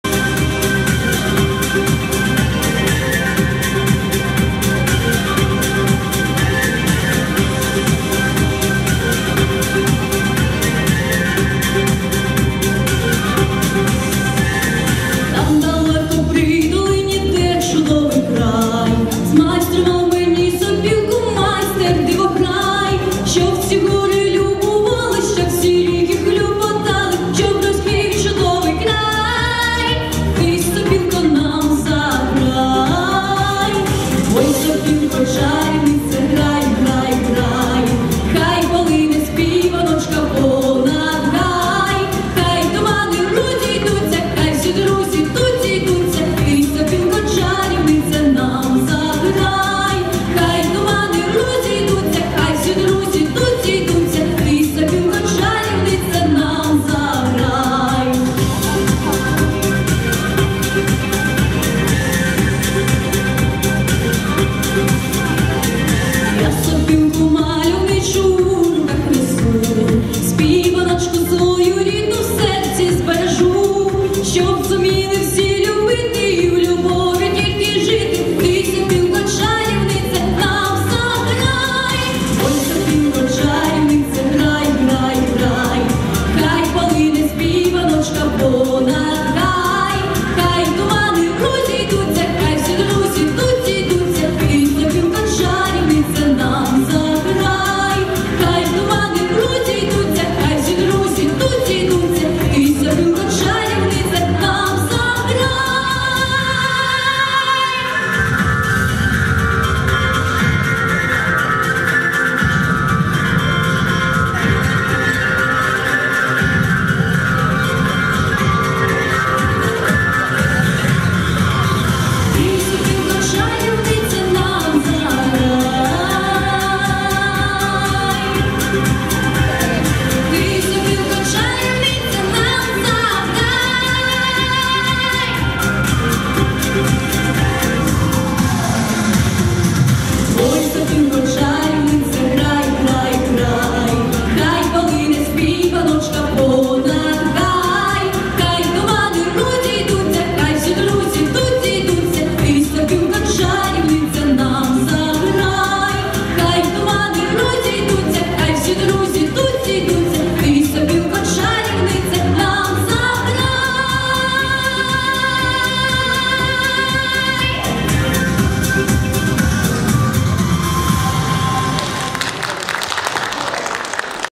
Гонтар Катерина Сопілка
Учасниця ОБЛАСНОГО ЕТАПУ ВСЕУКРАЇНСЬКОГО ФЕСТИВАЛЮ ДИТЯЧОЇ ТА ЮНАЦЬКОЇ ТВОРЧОСТІ «ЧИСТІ РОСИ», НОМІНАЦІЯ «ХУДОЖНЄ ВИКОНАВСТВО» – «СПІВУЧА ВЕСЕЛКА»
вихованка Зразкової вокальної студії «Перлина» Комунального позашкільного навчального закладу Тростянецької районної ради «Палац дітей та юнацтва», керівник Буднік Олена Сергіївна